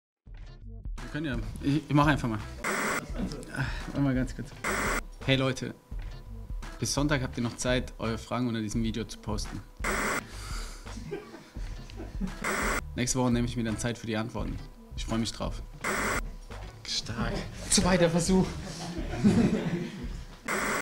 8unter99 - Frag' Kai
Nach Niklas Süle stellt sich dieses Mal Kai Herdling euren Fragen. Feuer frei!